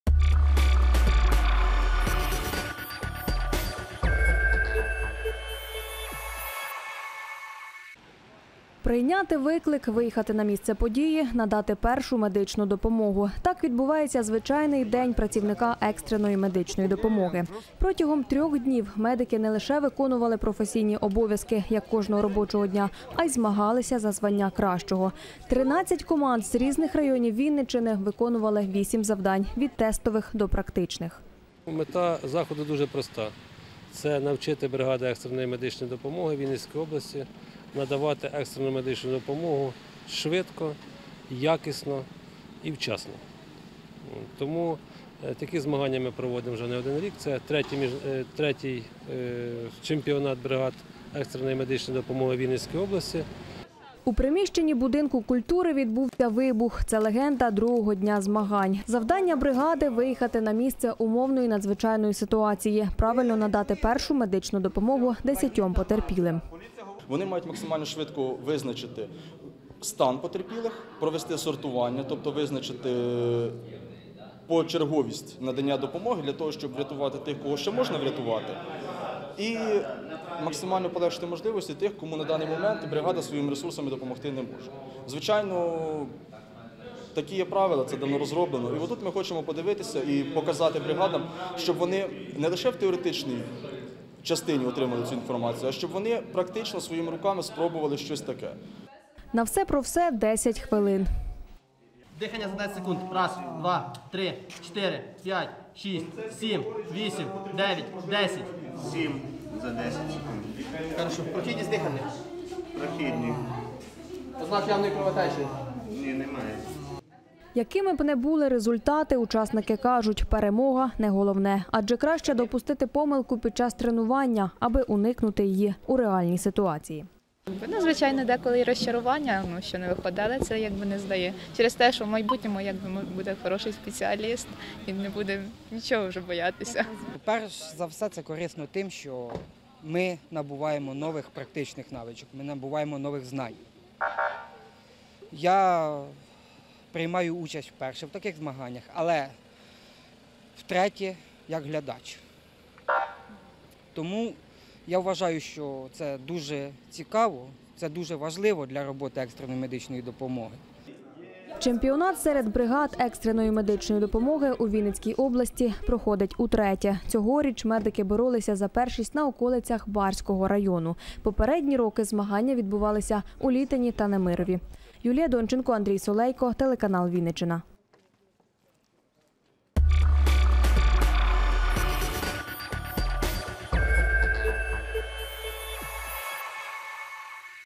Чемпіонат серед бригад екстреної медичної допомоги 07 09 2018
Здобути реальних навичок в умовних ситуаціях – одна з головних цілей чемпіонату серед бригад екстреної медичної допомоги. 3 дні команди медиків зі всієї області боролися за першість. Конкурси різних рівнів складності, завдання в будь-яку пору доби та мінімум відпочинку. Про особливий вид спорту надавачів першої медичної допомоги розкаже Юлія Донченко.